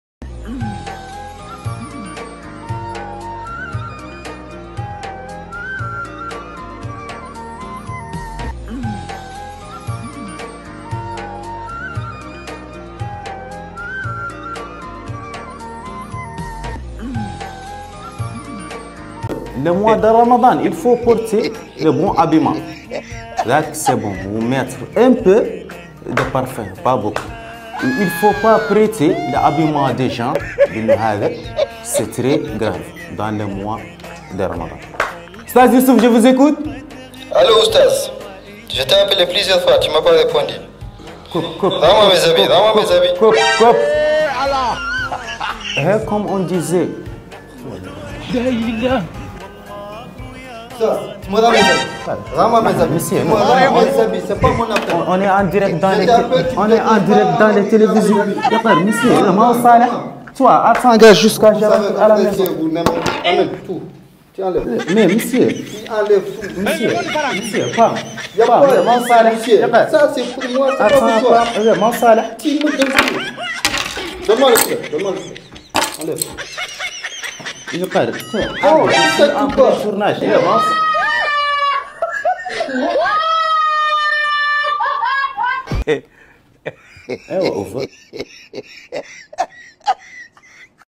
SAV RAMADAN DE OUSTAZE YOUSSOUF EP 6
Quand Oustaze prend un prêt pour un don !

Retrouvez tous les soirs sur sa chaîne TikTok et sur le YouTube de Wonibik TV ses aventures !